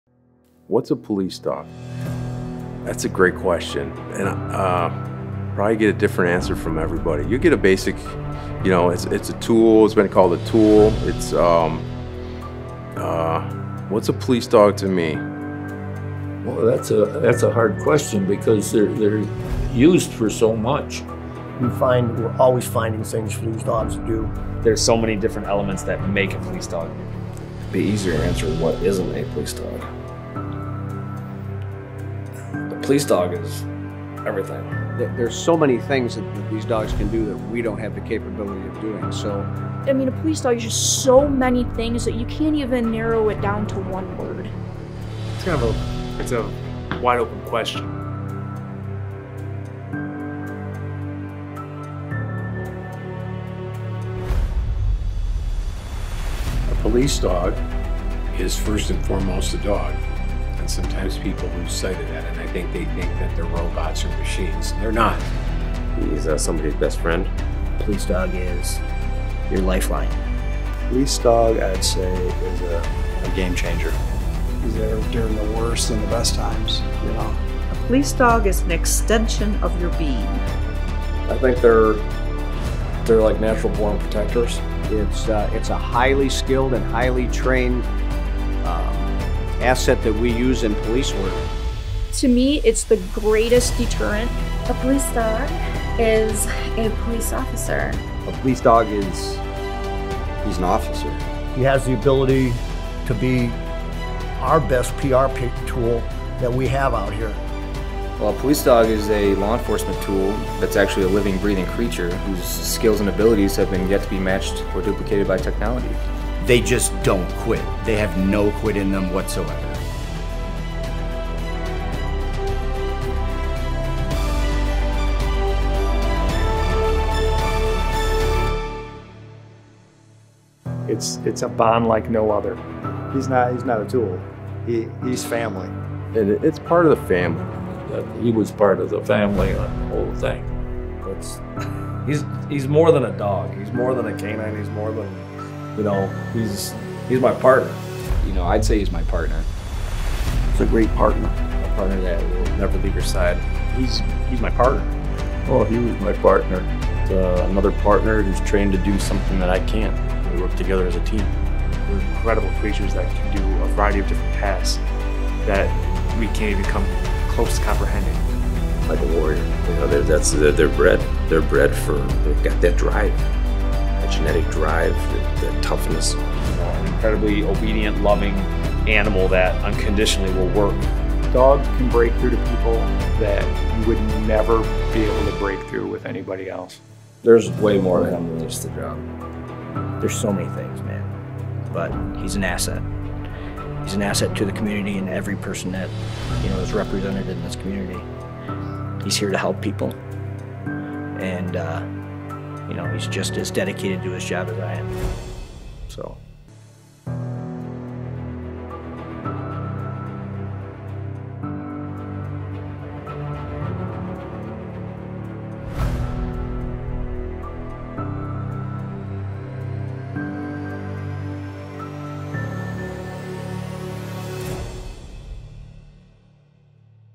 What Is A Police Dog?
It's a simple question with a complex answer: "What is a police dog?"
They're endlessly fascinating, with incredible skills and abilities.
Join us, as we explore this amazing branch of law enforcement, and see what it takes for a dog to become a police K9. Follow along with our documentary production of "Under The Vest: Inside the World of Police Dogs."
to learn more.